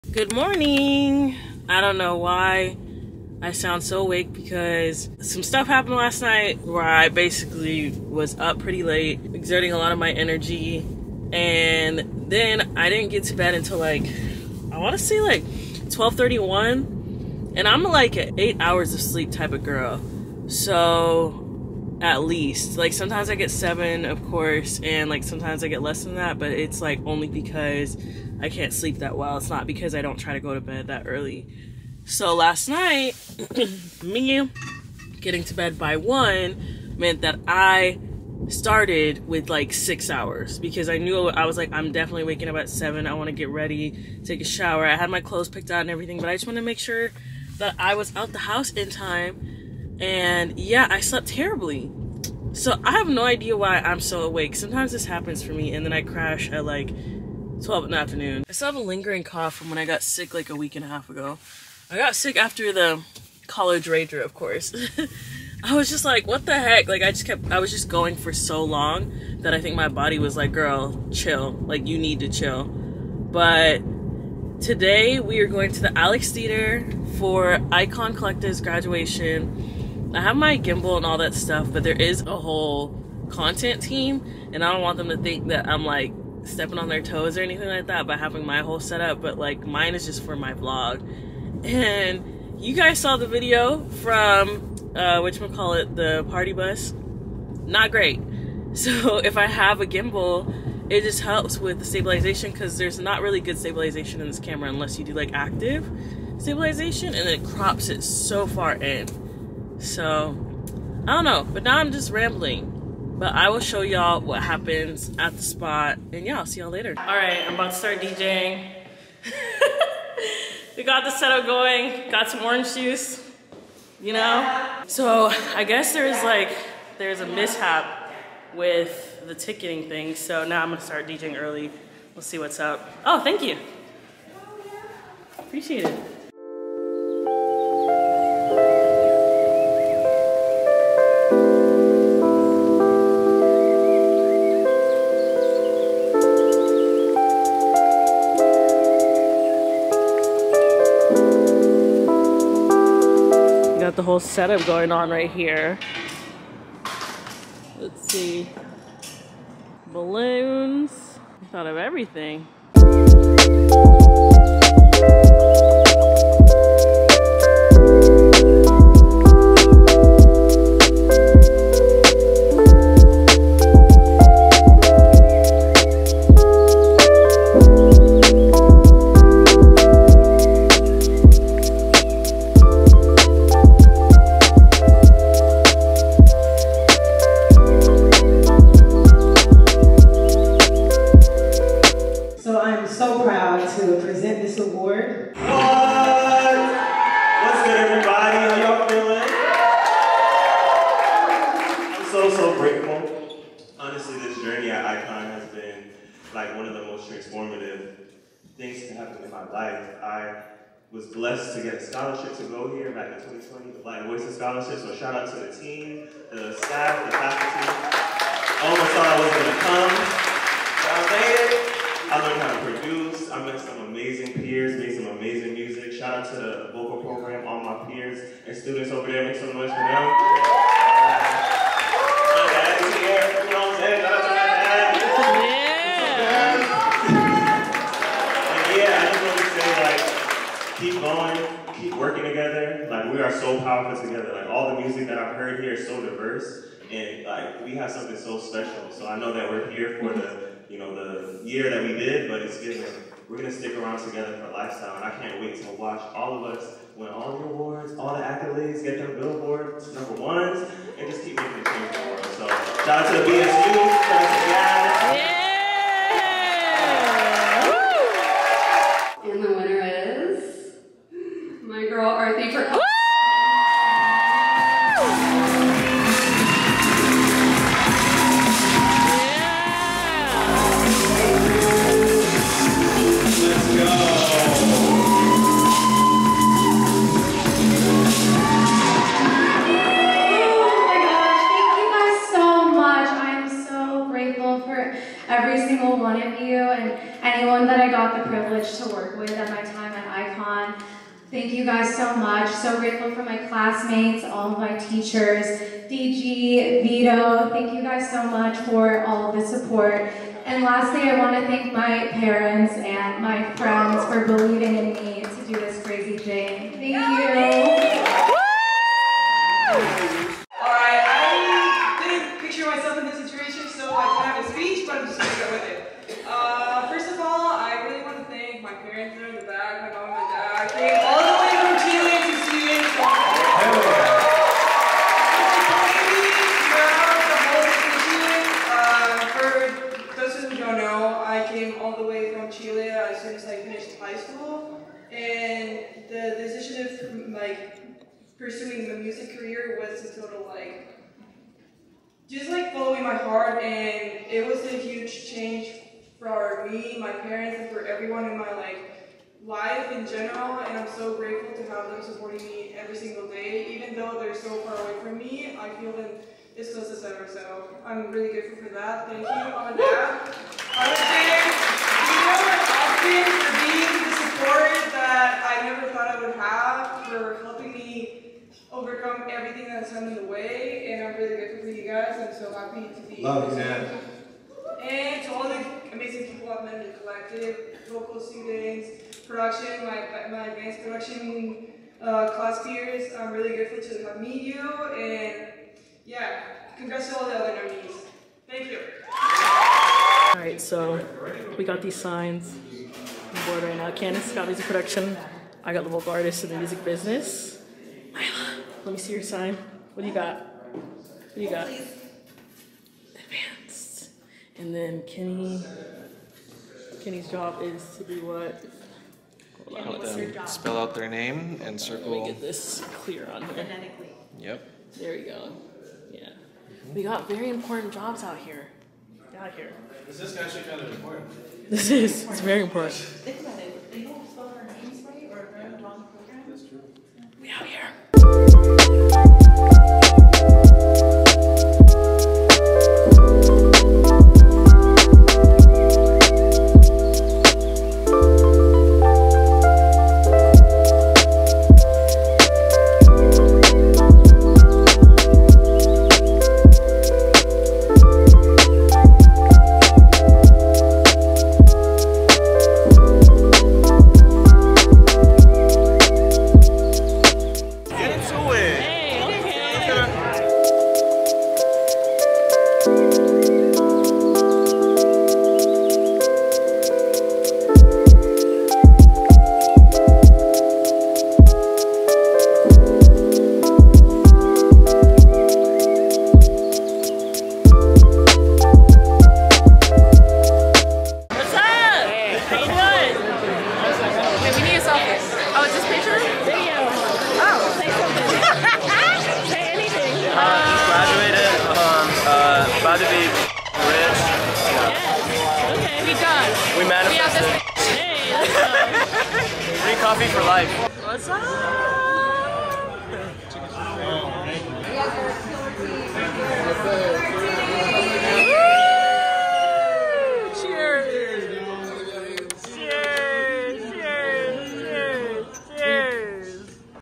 ICON Graduation Vlog: LA Music Production School Graduation
What's Good Y'all?!

So I work for Icon Collective (a music school in Los Angeles) and this past weekend was our graduation for all the grads of our programs. I was able to vlog the ceremony and all the new grads. It was a fun and exciting time and I am so grateful to work at a place where everyone treats you like family and we get to be around musicians of all walks of life.

00:00 - INTRO
01:57 - Pre-Graduation
03:07 - Ceremony
07:23 - Post-Graduation